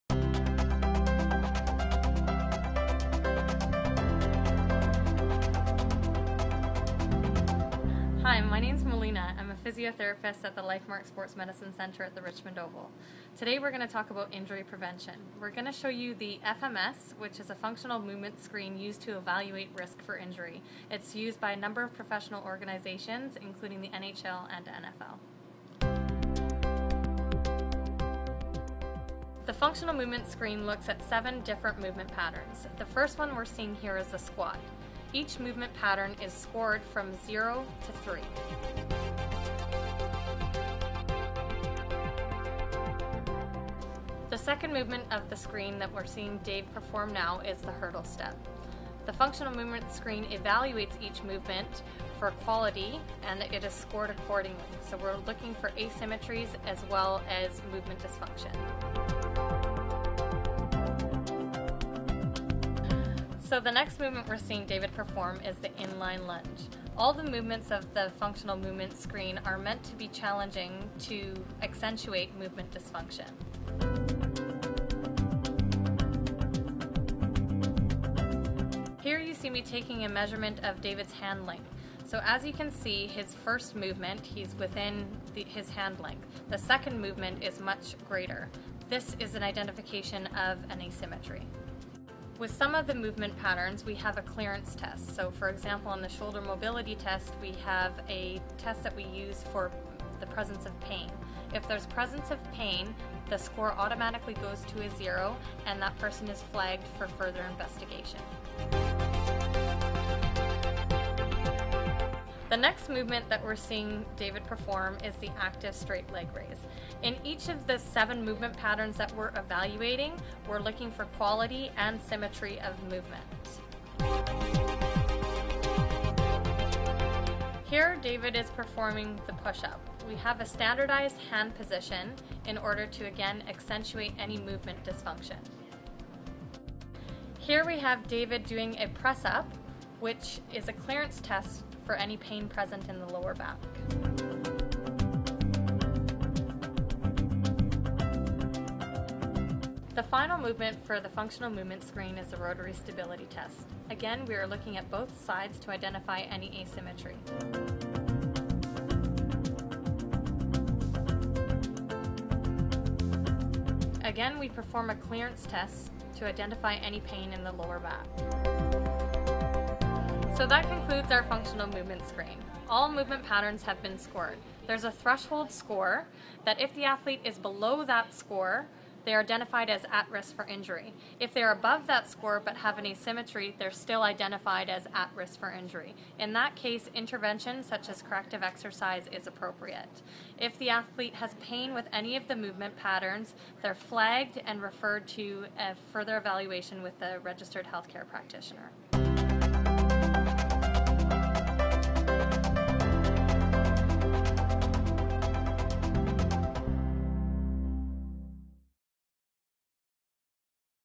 Functional Movement Screen
In this video, LifeMark Richmond Oval Clinic Director and Physiotherapist, Melina Kurtakis demonstrates the process for conducting a Functional Movement Screen, a testing method used by leading organizations like the NHL and NFL to detect movement instabilities and asymmetries to help athletes train more effectively and prevent injuries.